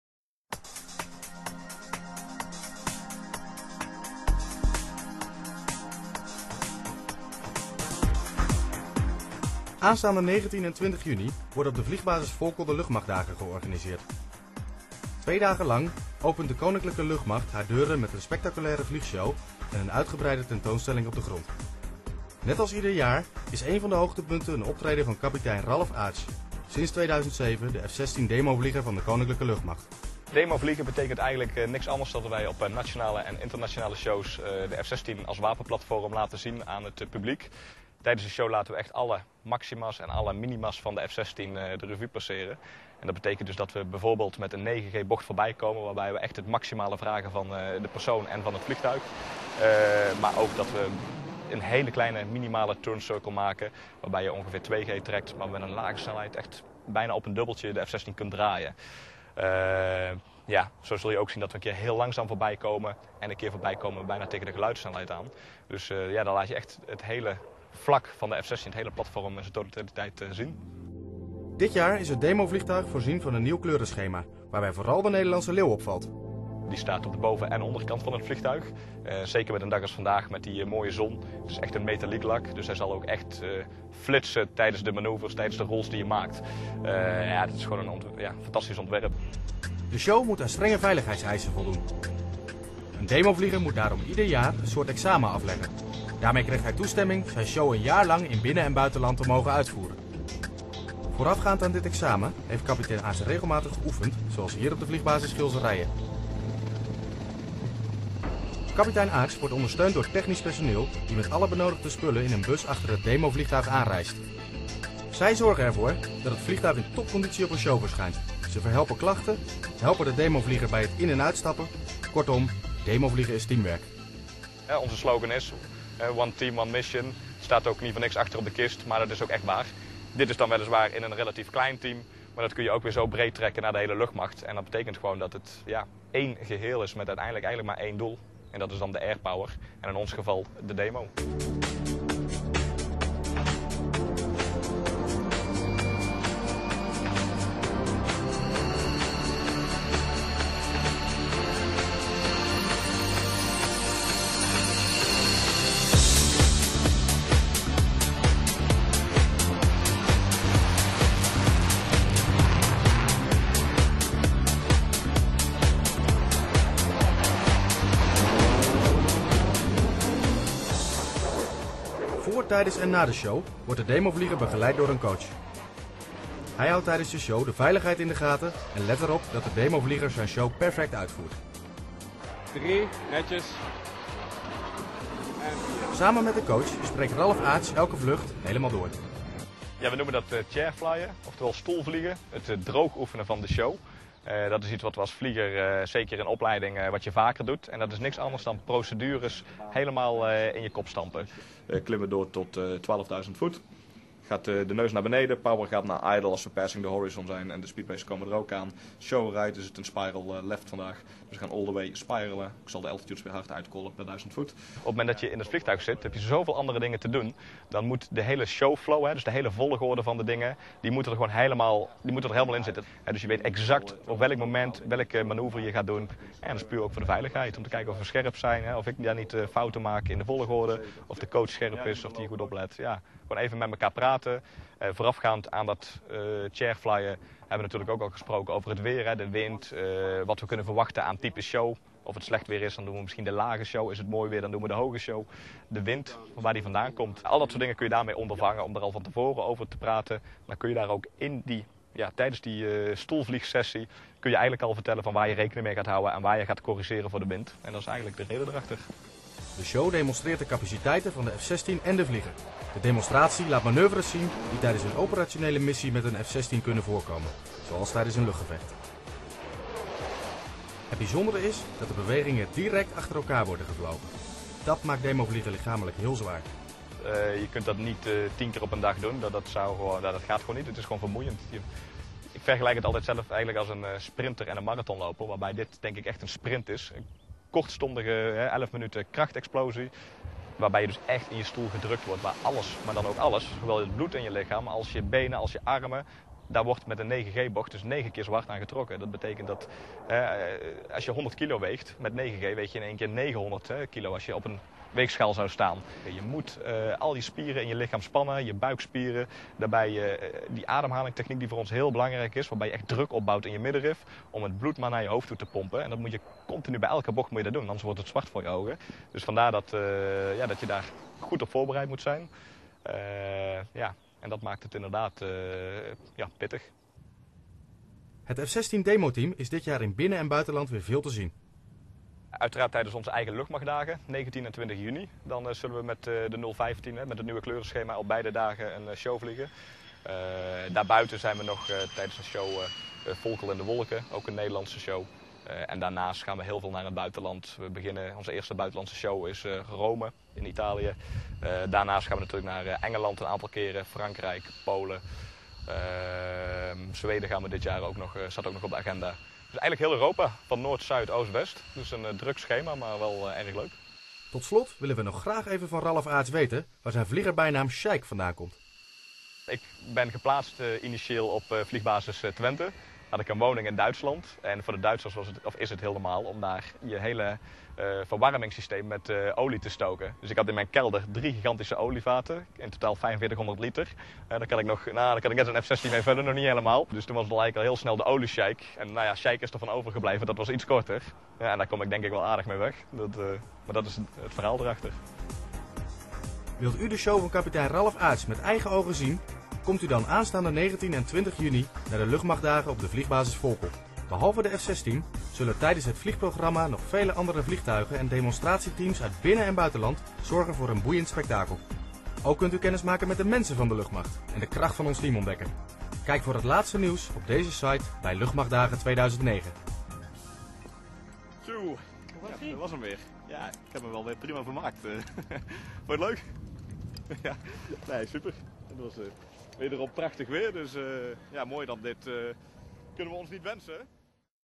Demo F-16 Royal Netherlands Air Force Display
Special on this F-16 display is that all manoeuvres are flown one after the other going smoothly one into the other. This F-16 Solo Display makes part out of the presentation of the Royal Dutch Air Force for pilot recruitment. I will have to find some time to make the subtitles.